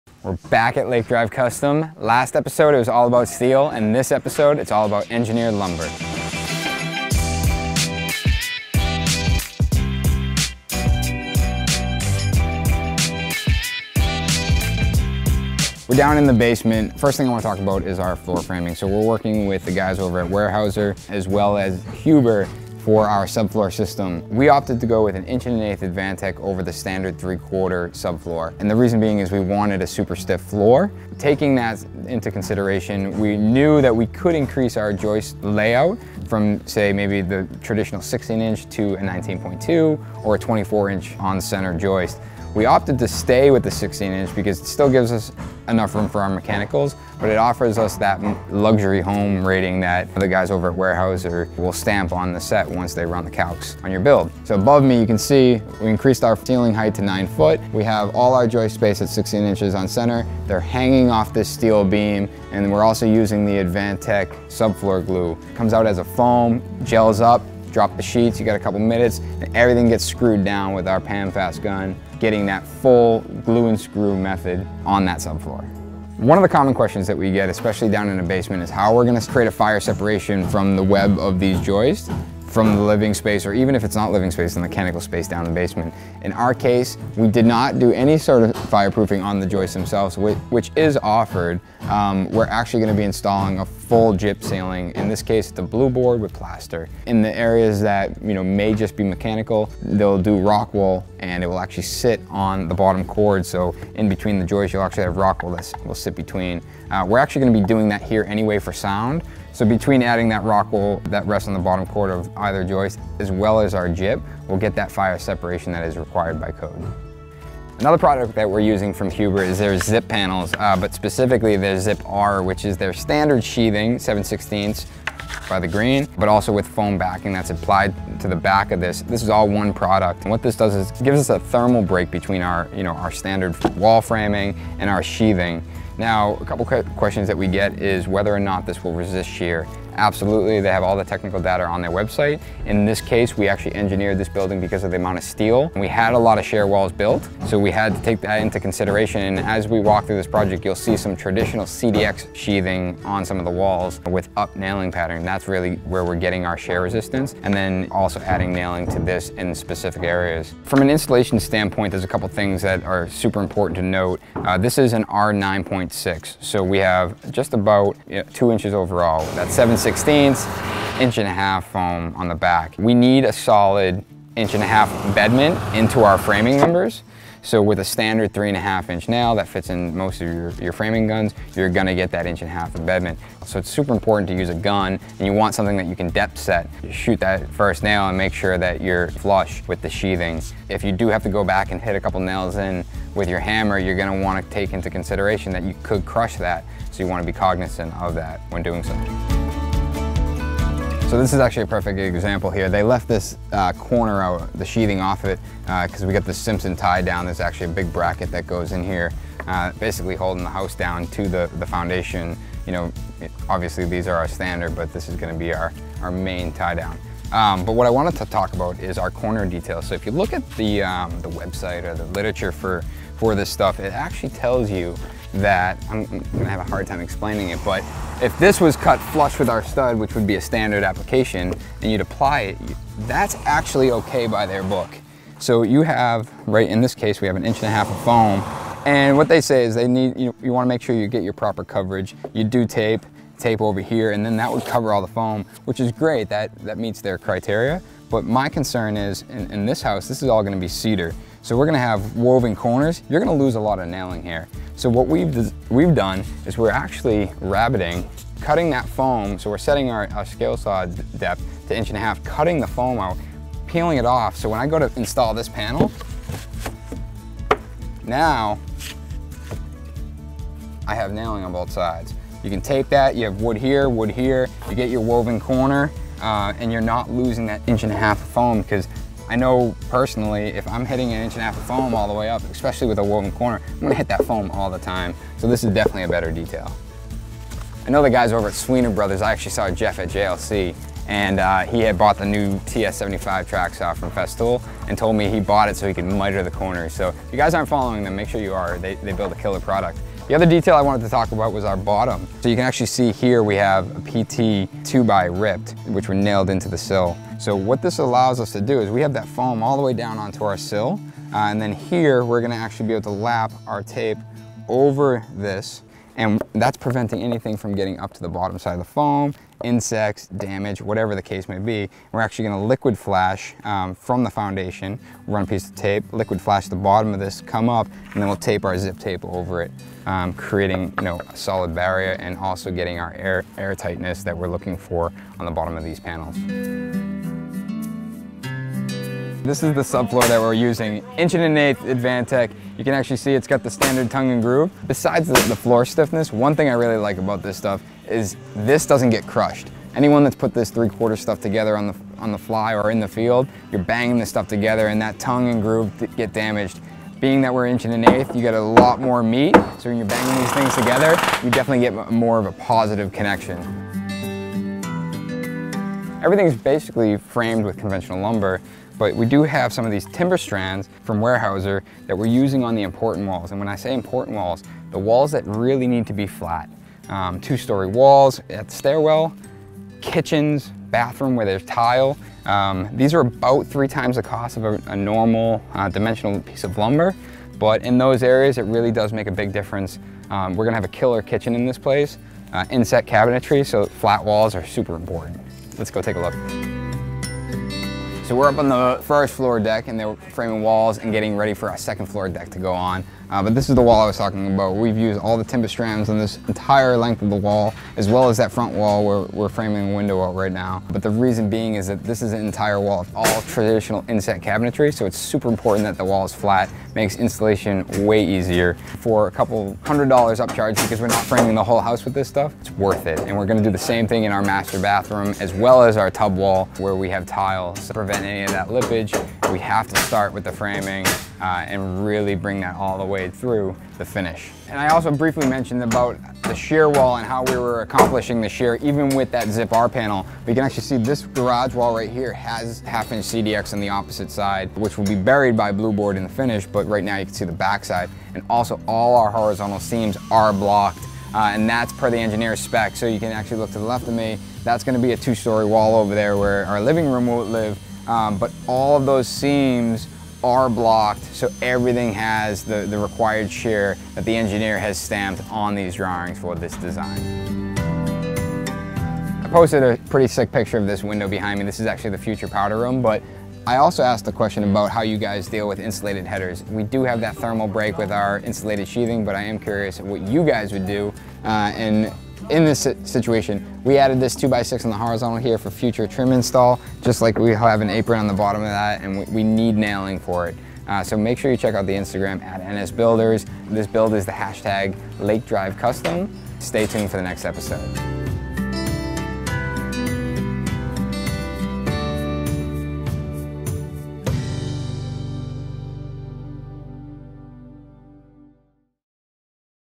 Engineered Wood: Lake Drive Custom: Ep 03
Have you heard of Huber?
What about Zip Sheathing and Advantech Subfloor? Huber is the manufacturer of these two products and on this build we're using both.
Standard subfloor is 3/4" but we've upped it to 1-1/8"!
Instead of traditional sheathing we're using the Zip R-sheathing, R9.6 to be exact. A sidewall sheathing with a foam insulation backer giving us a full thermally broken exterior wall. Watch the video and find out the benefits, best practices and key points on this build.

Also discussed in this build is the Weyerhauser i-joists and Timerstrand engineered studs, two products where using. One of them landing us a Luxury Home pro rating.

Follow the build on Instagram

How do we manage our projects from the big picture all the way down to the smallest detail?

Lumber being provide by
Riverhead Building Supply @riverhead_building_supply